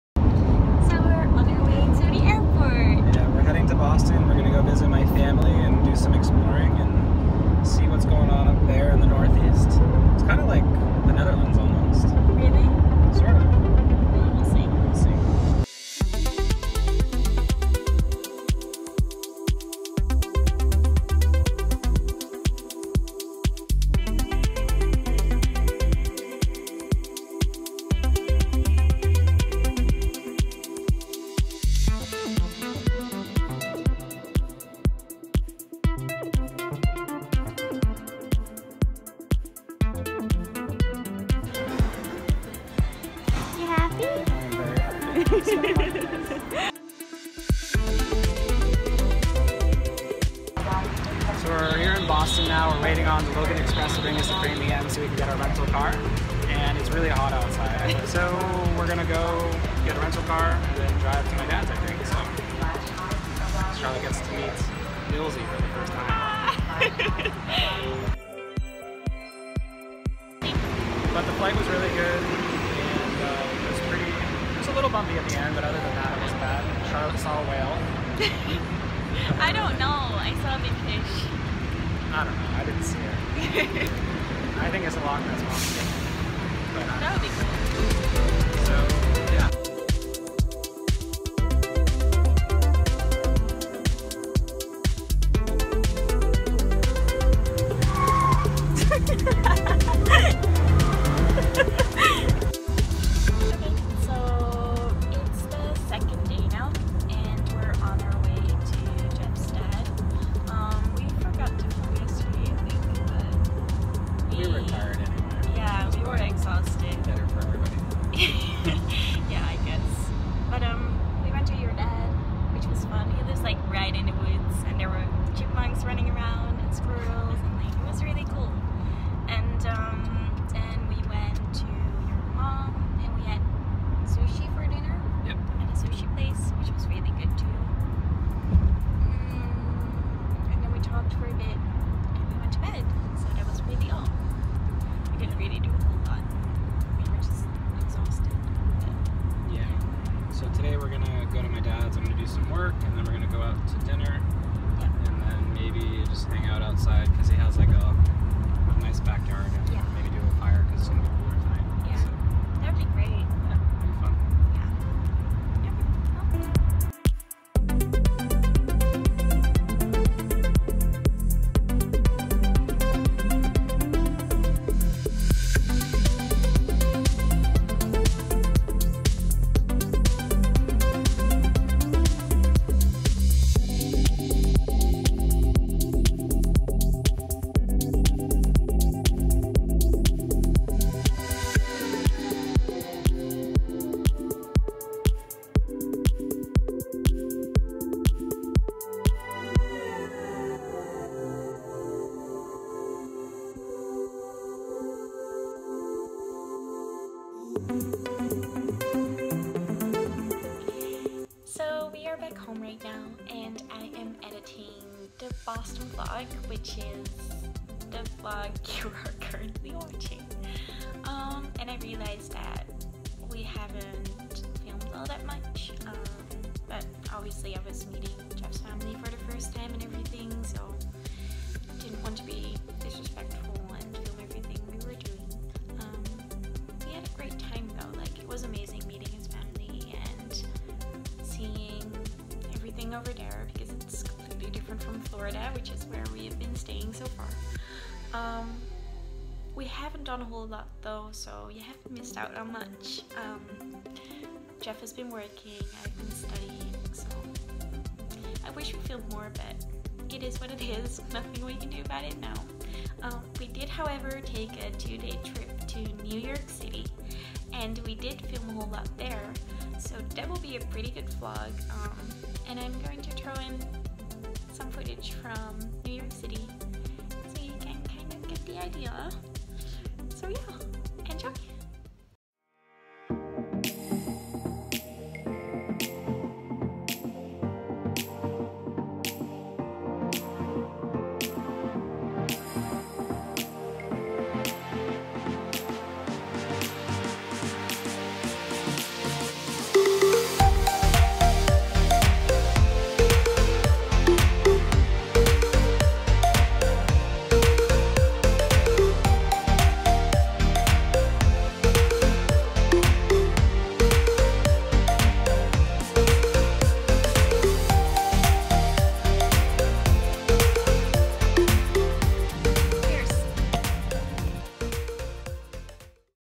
Boston | Vlog | LifeAcrossOceans
We've spend about a week in Boston Massachusetts to meet Jeff's family.